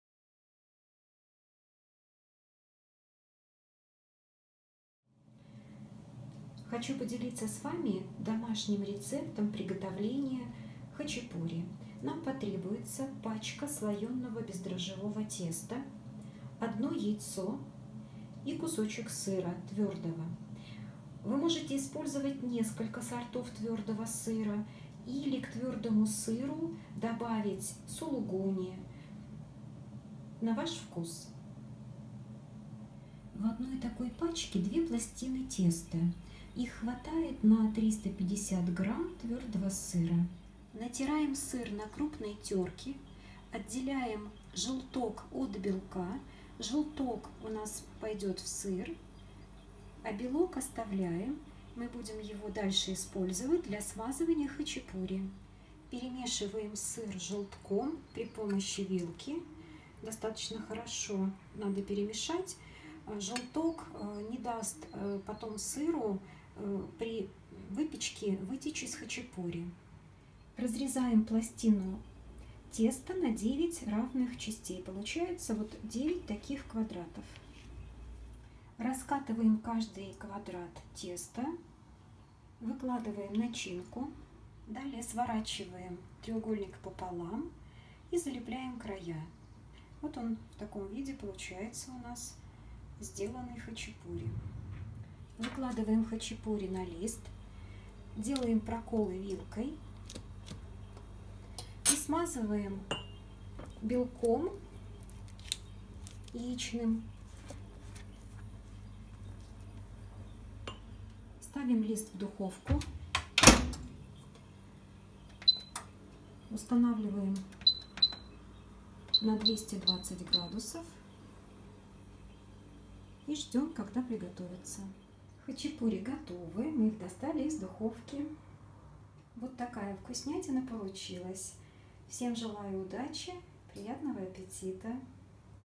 Хачапури с сыром из слоеного теста / Cheese filled puff pastries
Видео домашнего рецепта о том, как просто, быстро и вкусно приготовить хачапури с сыром в духовке.